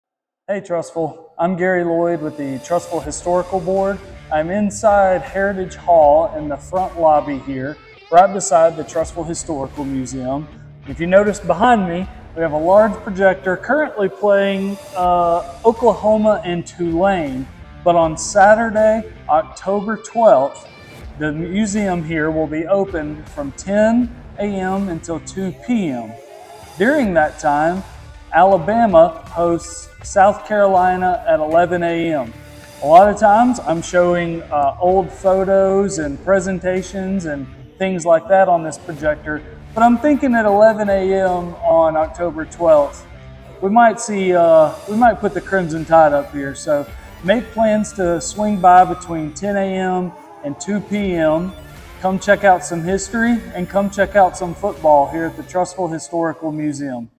Alabama football viewing party at the city historical museum?
Come out to the Trussville Historical Museum on Saturday, Oct. 12, 2024, from 10 a.m. to 2 p.m. and watch Alabama take on South Carolina with us! Peruse the Trussville Historical Society's Trussville-themed souvenirs, learn some local history, and watch the Tide roll!